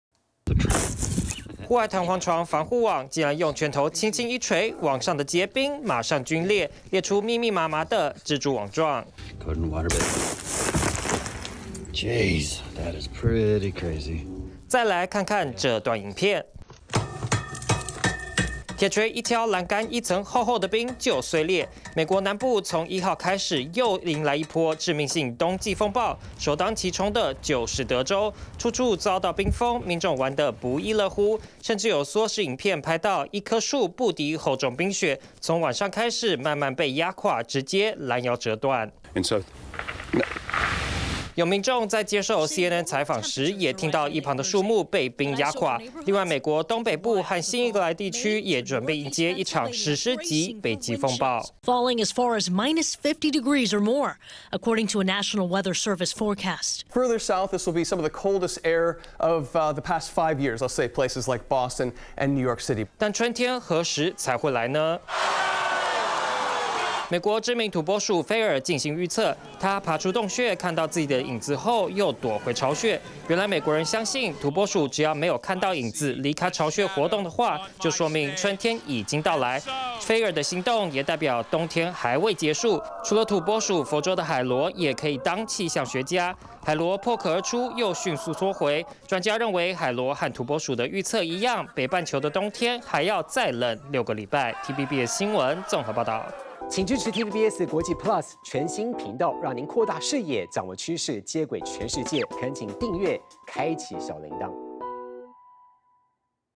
護網.欄杆都結冰 美國德州人「敲冰趣」 土撥鼠預測:再冷六周｜TVBS新聞
美國2月開始，又迎來另一場冬季風暴！尤其是在德州，因為氣溫實在太低，有民眾把彈簧床放在後院，沒想到連護網都冰凍到結冰。而且部分路樹，因為承受不住冰晶的重量，攔腰折斷。現在雖然風暴已經慢慢減弱，不過美國氣象單位警告，接下來輪到美國東北，可能出現零下50度的史詩型風暴。